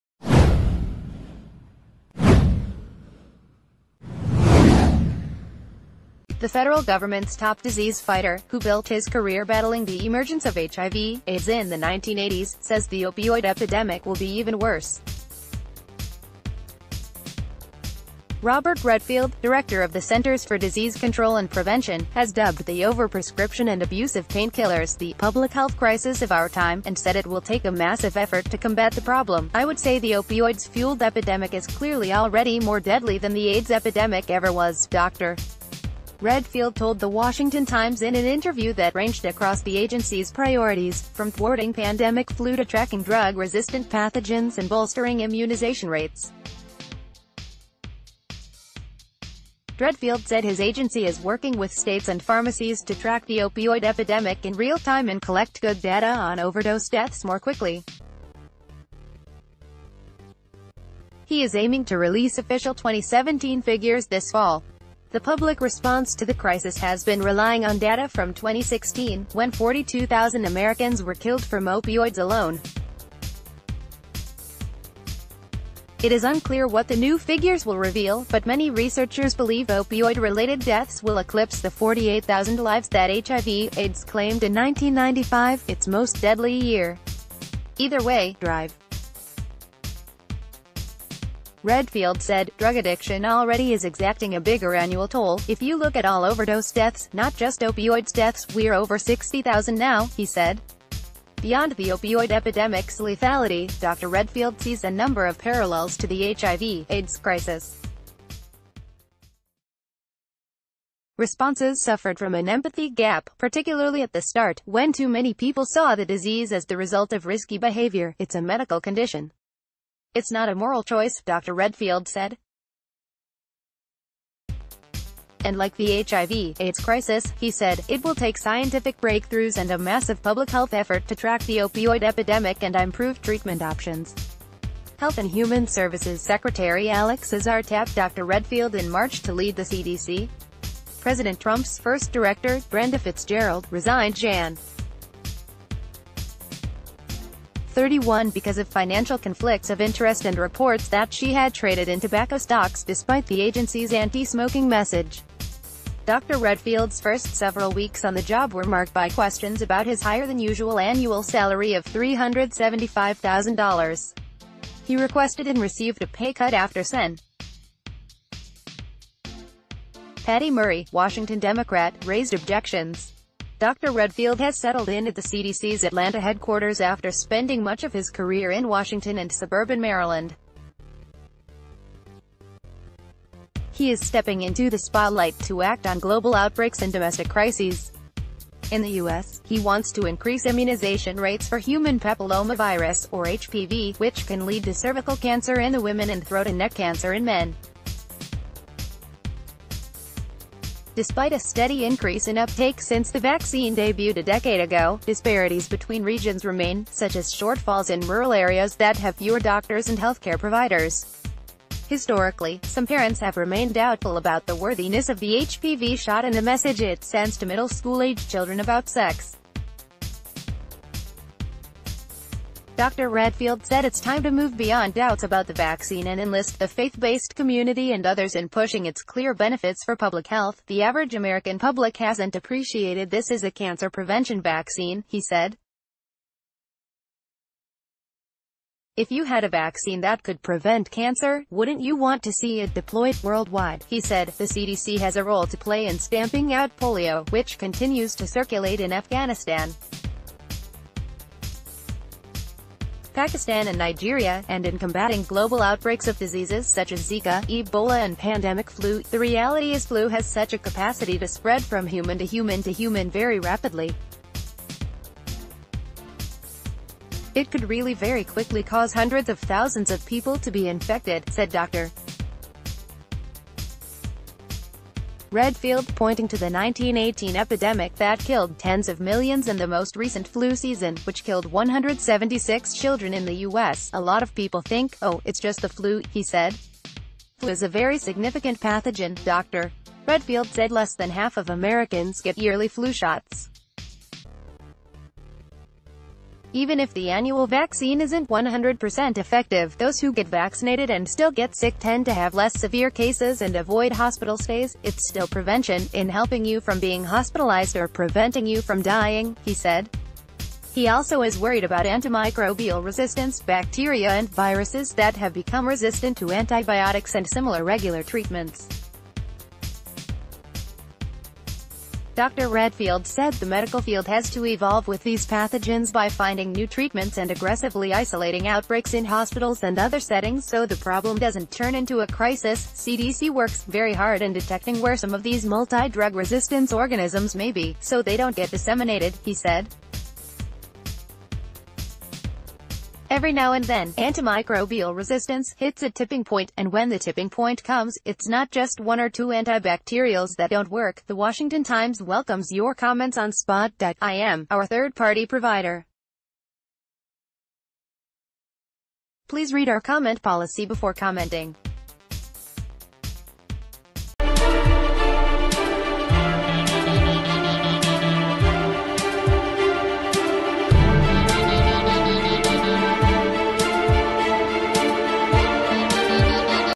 Robert Redfield, CDC director: Opioid epidemic worse than AIDS
The federal government’s top disease fighter, who built his career battling the emergence of HIV/AIDS in the 1980s, says the opioid epidemic will be even worse. Robert Redfield, director of the Centers for Disease Control and Prevention, has dubbed the overprescription and abuse of painkillers the “public health crisis of our time” and said it will take a massive effort to combat the problem.“I would say the opioids-fueled epidemic is clearly already more deadly than the AIDS epidemic ever was,”...